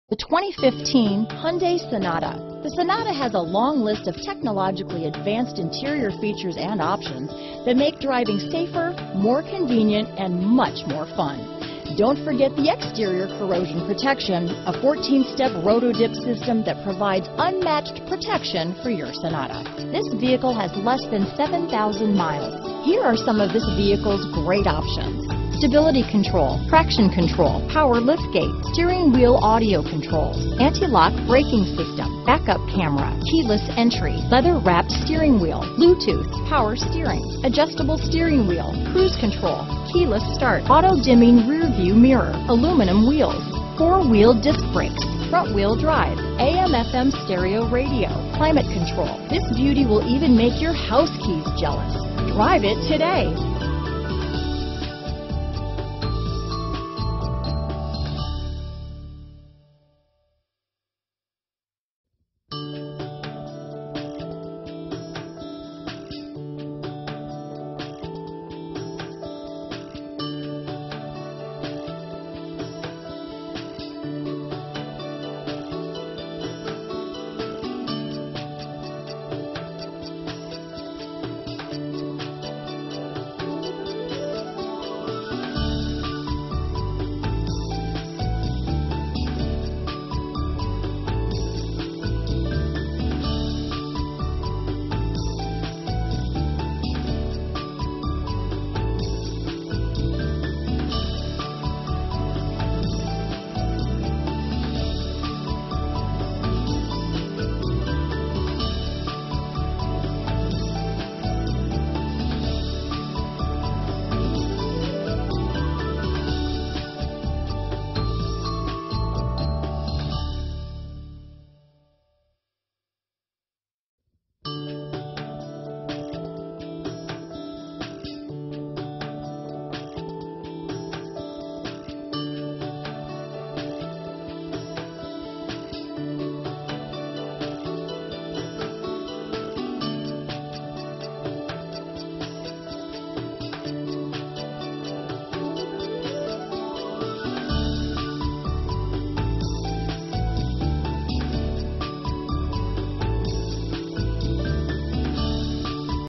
2015 Hyundai Sonata Aurora, Denver, Highland Ranch, Parker, Centennial, CO 35171A
2015 Hyundai Sonata
2015 Hyundai Sonata Limited 2.0T - Stock#: 35171A - VIN#: 5NPE34AB0FH184145

Mile High Honda is pleased to offer this 2015 Hyundai Sonata 2.0T Limited, Venetian Red and Gray interior, LOW MILEAGE, NAVIGATION/GPS/NAV, LEATHER, ONE OWNER, and ACCIDENT FREE CARFAX.Who could say no to a truly wonderful car like this beautiful 2015 Hyundai Sonata? What a perfect match! This superb Hyundai Sonata is available at the just right price for the just right person - You!All prices include Dealer Handling fee of $499. Call us with any questions.